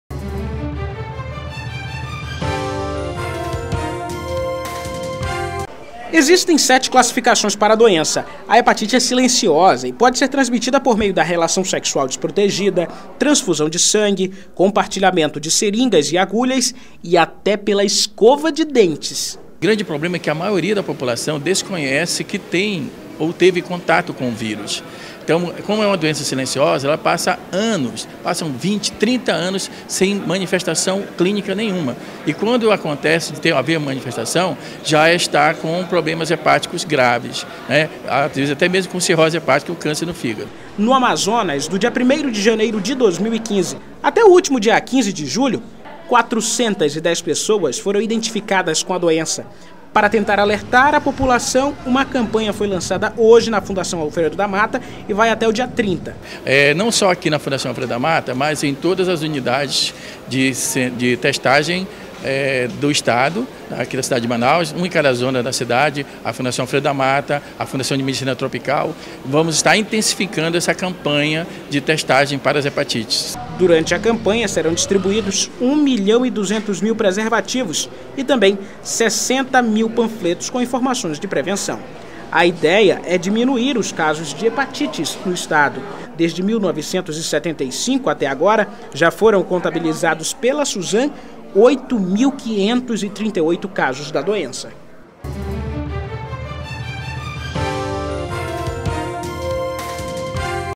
Dia Mundial de Combate a Hepatite é lembrado em Manaus com campanha
Nesta terça-feira, 28, é o Dia Mundial de Combate à Hepatite. E para chamar a atenção da população começou em Manaus uma campanha de incentivo a prevenção, diagnóstico e tratamento da doença. Nos primeiros cinco meses do ano foram registrados mais de quatrocentos novos casos de hepatite no estado.